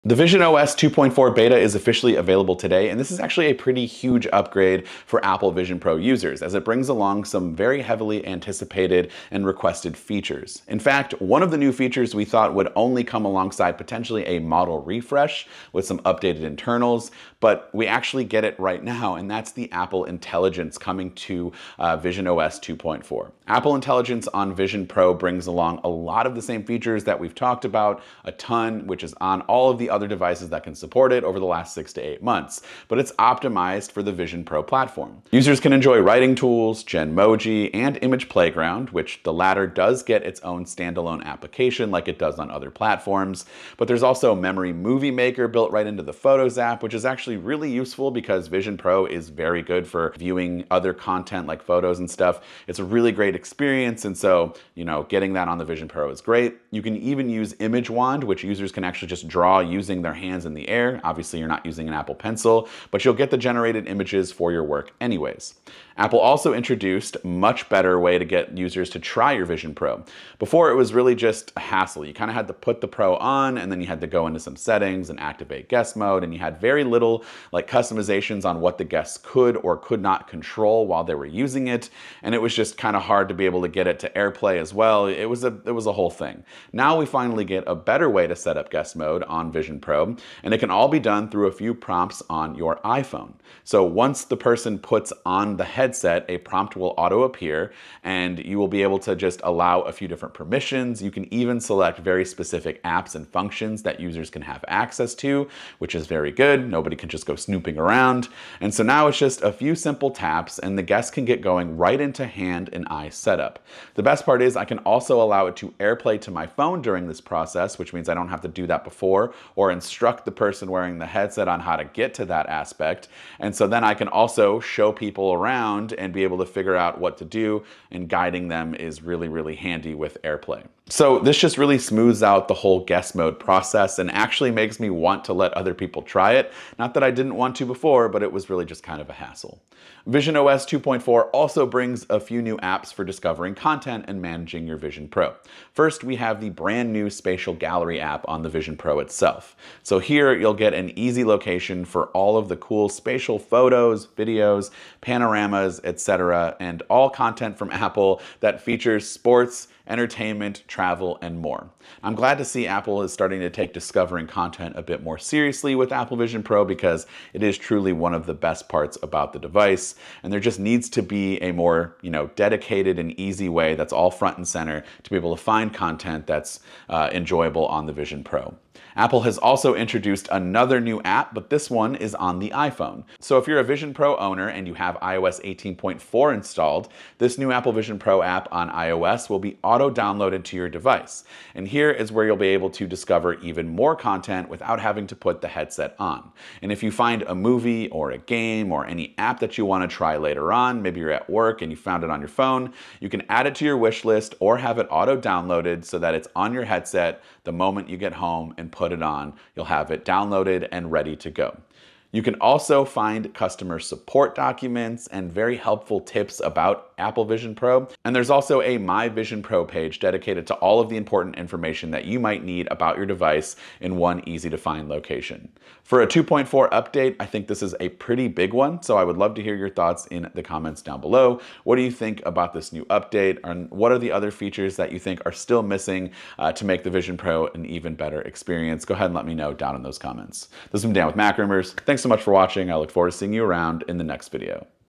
visionOS 2.4 Beta Out Now! A Big Update to Apple Vision Pro!
Apple today seeded the first beta of an upcoming visionOS 2.4 update to developers for testing purposes, with the beta coming almost a month after the launch of visionOS 2.3.

visionOS 2.4 will be one of the most significant visionOS updates to date, because it brings Apple Intelligence to the Vision Pro for the first time. Apple started rolling out ‌Apple Intelligence‌ features on the iPhone, iPad, and Mac last year, but the Vision Pro was not included until now.

⏱️ CHAPTERS
00:00